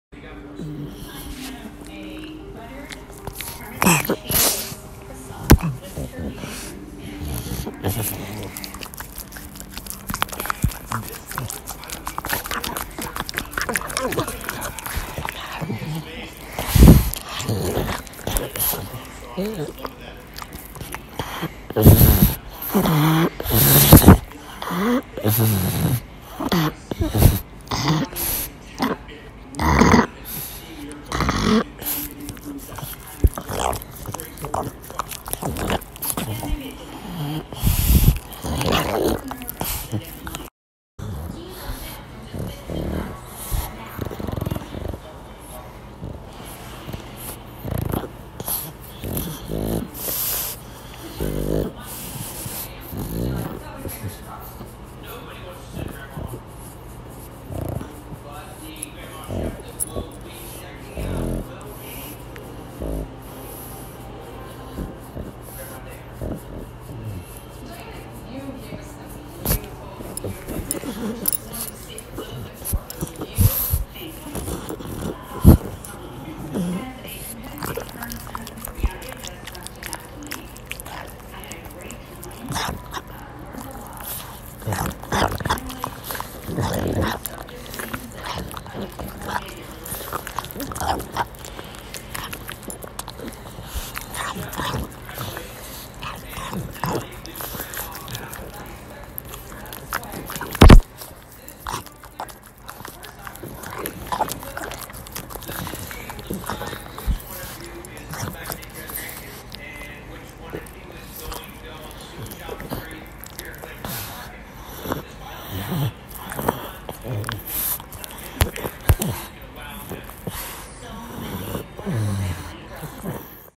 Pug ASMR Soft Sounds
This video was created with the intention of showing that ASMR is cringey and should not be on the internet